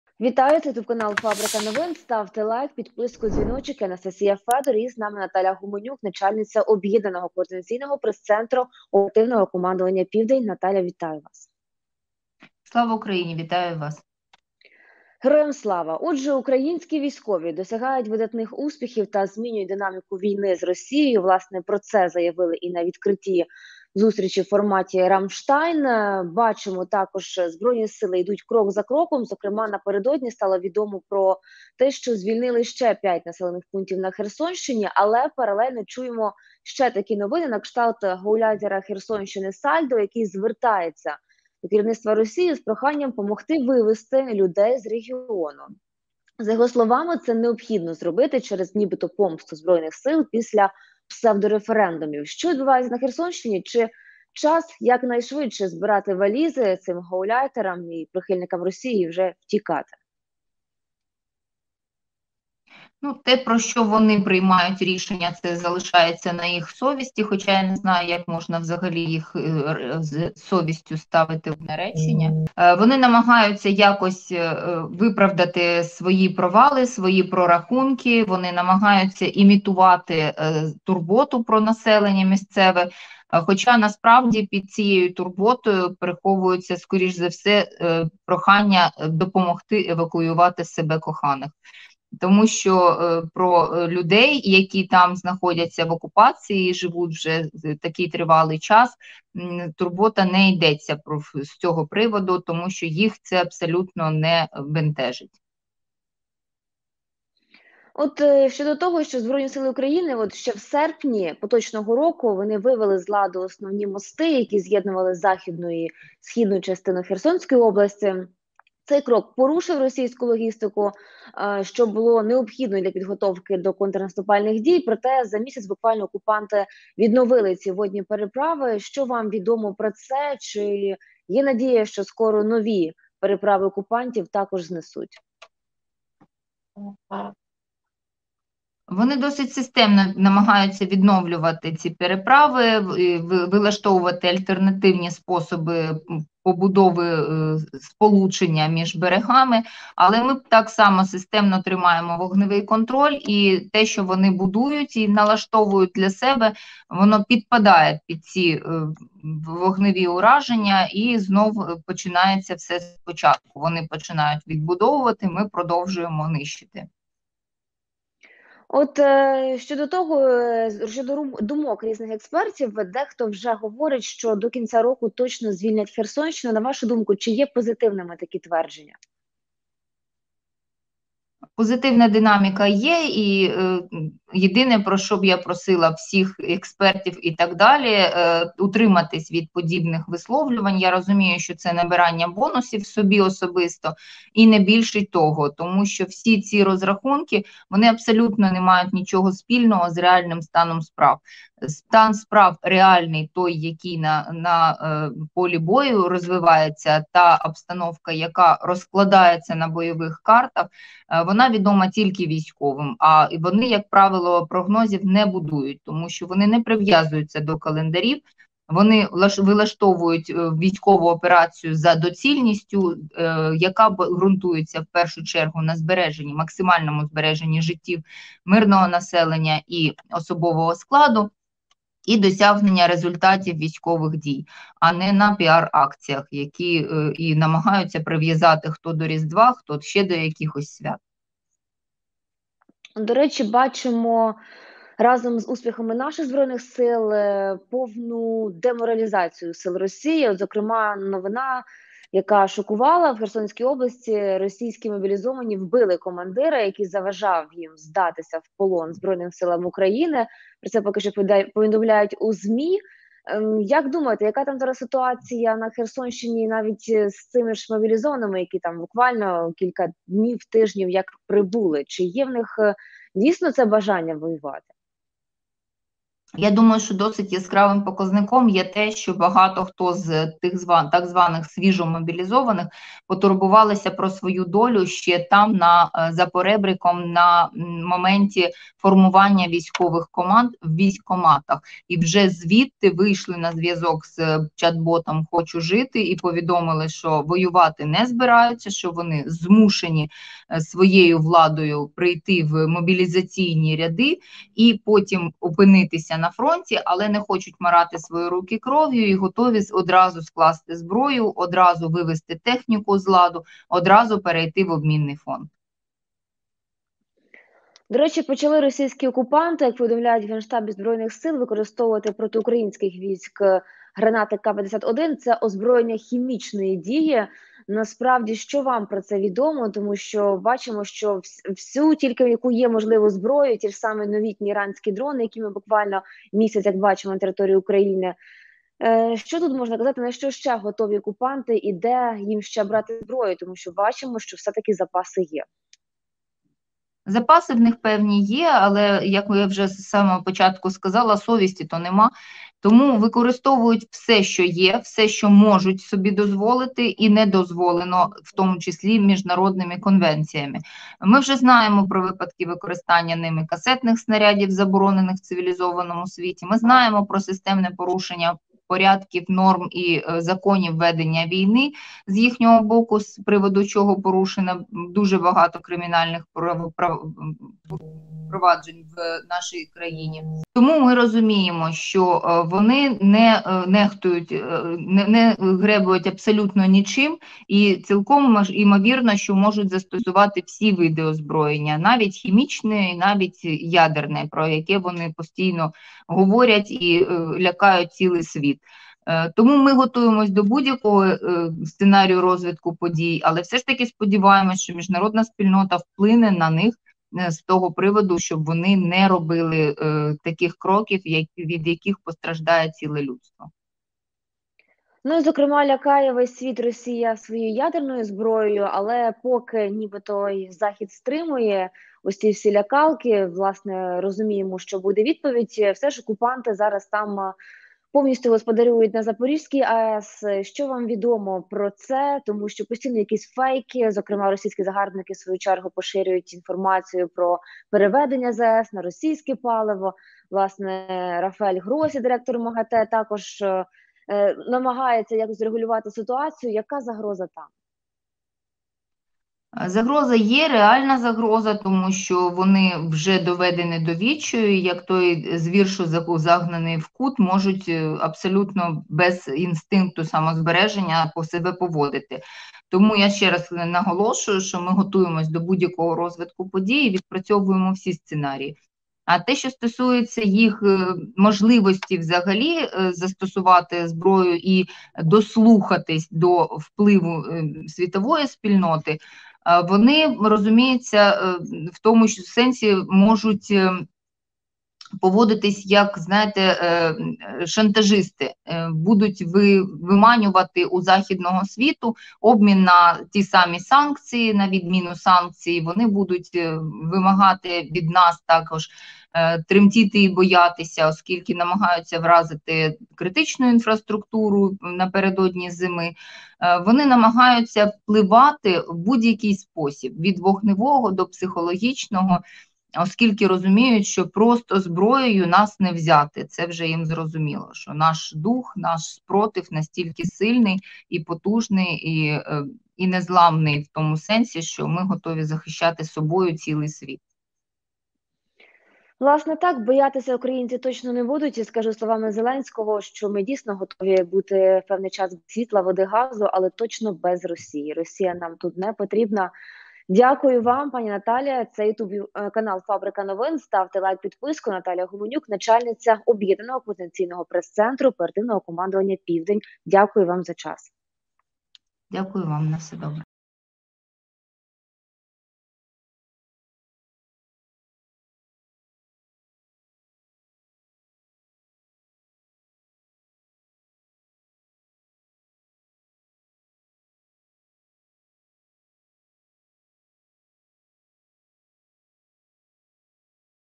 ⚡ГУМЕНЮК: термінове звернення Сальдо до Путіна, евакуація Херсону, мобіки тікають до ЗСУ
Начальниця об’єднанного координаційного прес-центру Оперативного командування «Південь» Наталія Гуменюк в інтерв’ю Анастасії Федор для «Фабрики новин» розшифрувала правду про евакуацію Сальдо, розповіла, як мобілізовані росіяни ще з РФ домовляються про полон. Також вона пояснила ситуацію із звільненням Херсону.